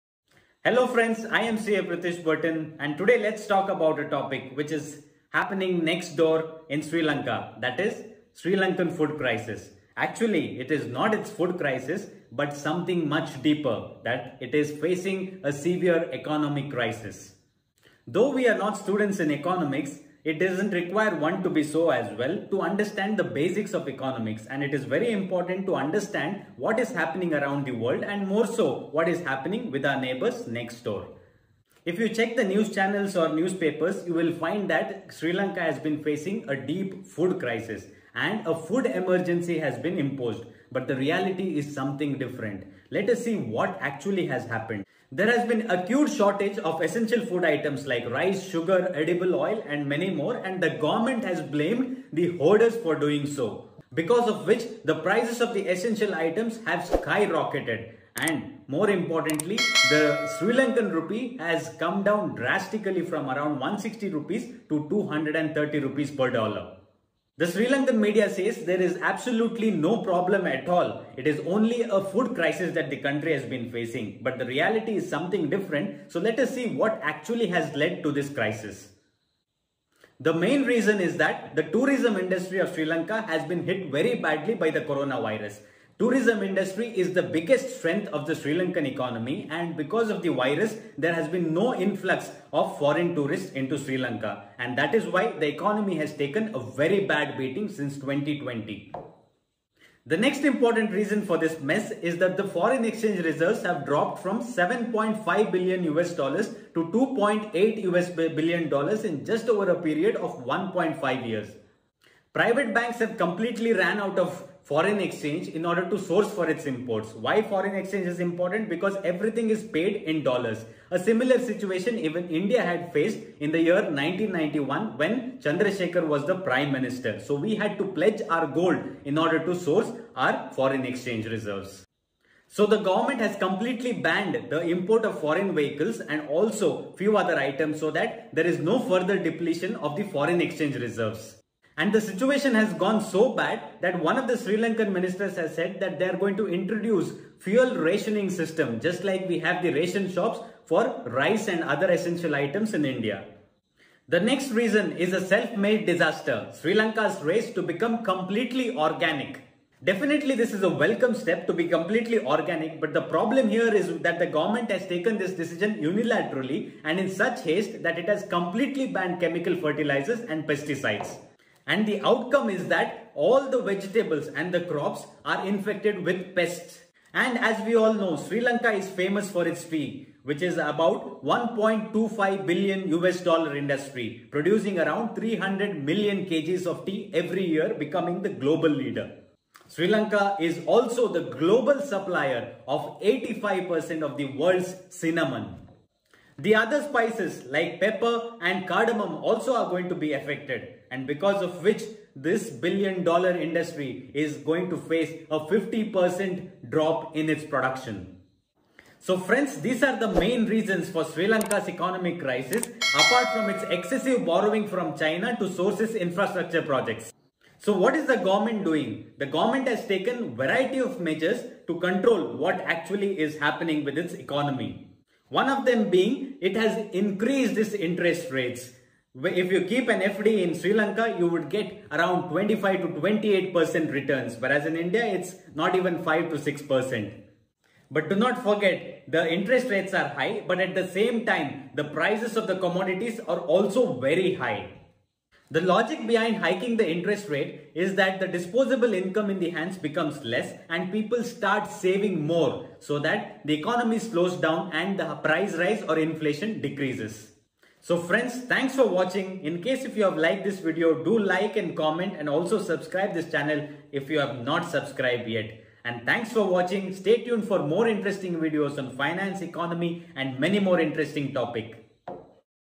ECONOMIC CRISIS IN SRI LANKA | FOOD EMERGENCY | CA PRITISH BURTON
Sri Lanka has been facing a severe economic crisis. Though it is termed as food crisis it is more to do with meltdown in the economy. The emergency state of economy is due to dwindling foreign exchange reserves, high inflation, acute shortage of essential food items, rising debt and a hasty decision to go completely organic by imposing a complete ban on import of pesticides and chemical fertilisers. Check out this complete video to know more in detail.